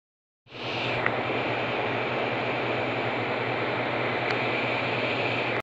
The reason why I haven't made videos is because I got in to a fire accident and got burnt...
Just give this 2 likes